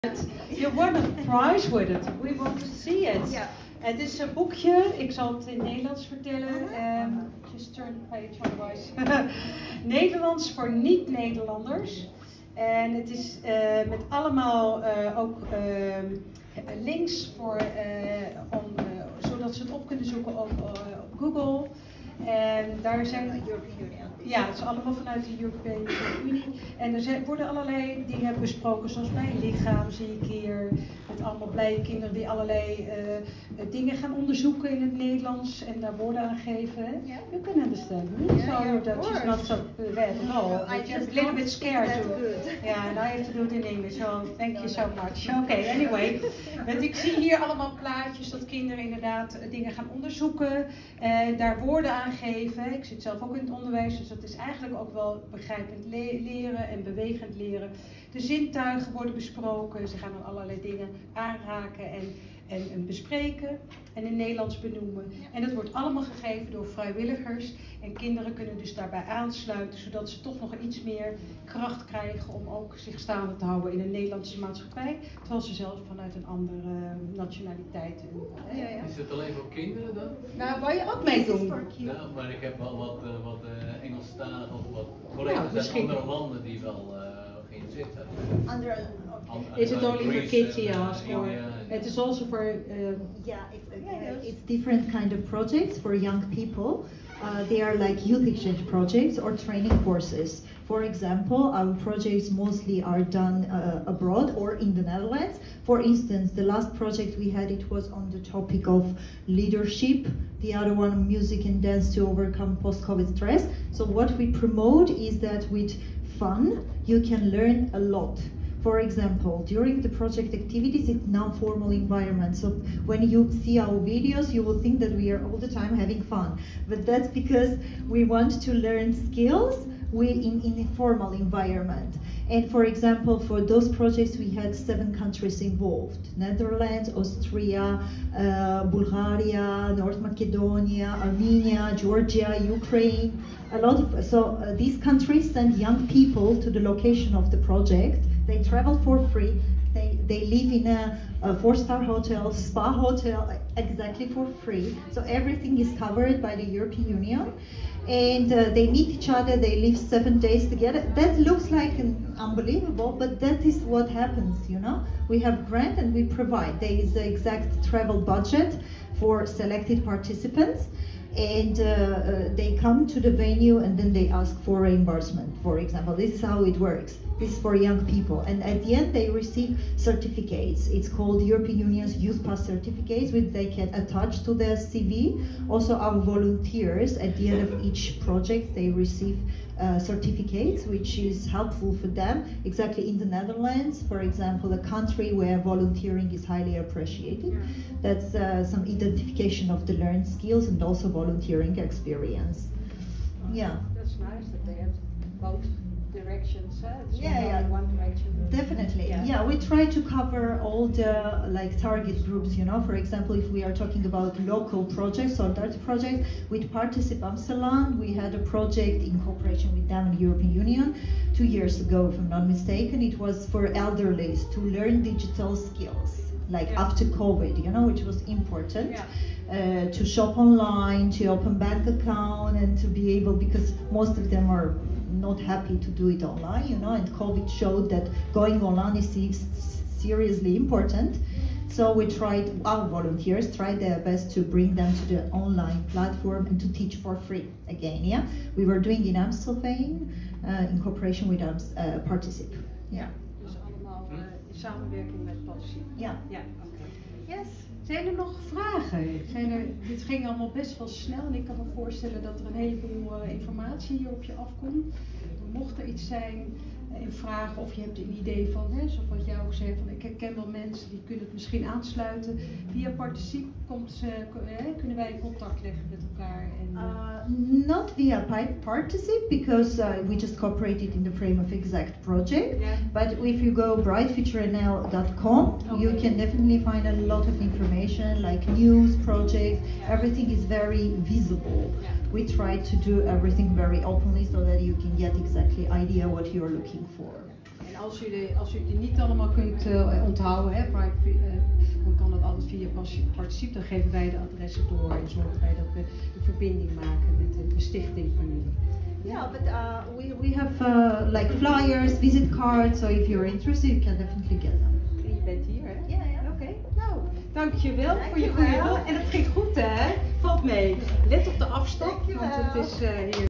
About our Youth Exchanges and Training Courses. Aalsmeer's Municipality Community Event.
On March 8 Bright Future contributed to the International Women's day big community event in Aalsmeer.

We presented our projects, donors, shared information on all the learning and volunteering opportunities our target groups and new team members/volunteers can find at Bright Future.

The event was organized by Participe in cooperation with Aalsmeer Municipality.

🥳Would you like to join us as a volunteer?

💖 Keep an eye on Bright Future's projects and events.